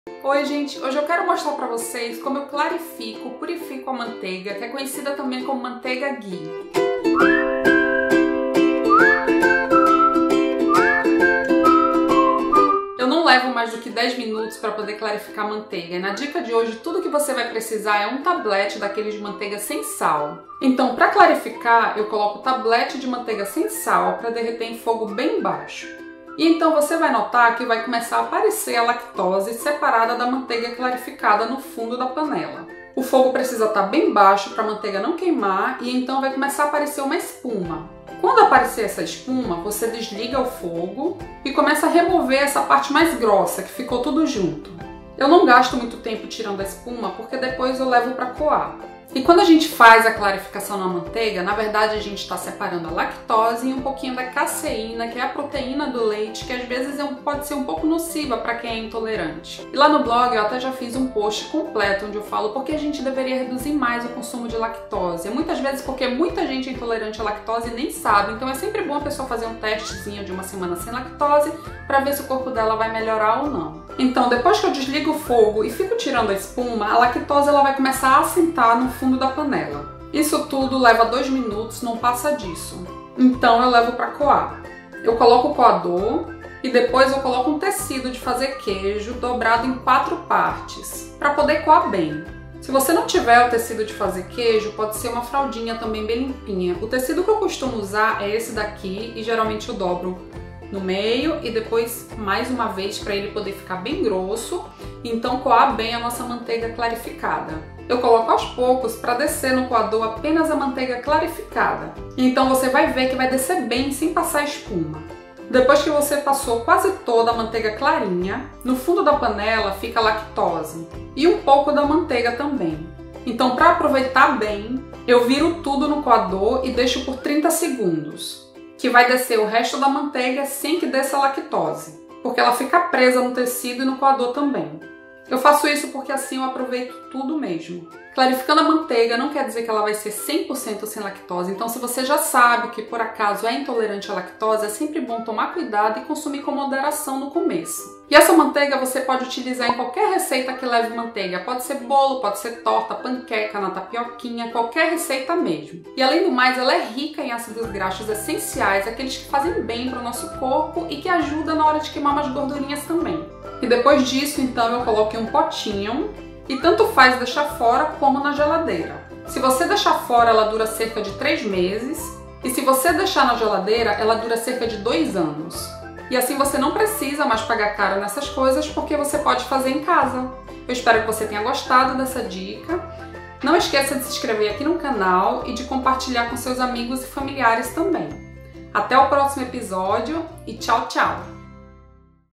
Como clarificar a manteiga | Manteiga ghee
Nesse vídeo eu ensino como clarificar a manteiga separando a lactose para utilizar em suas receitas favoritas, assim você vai poder usar manteiga sem sentir culpa.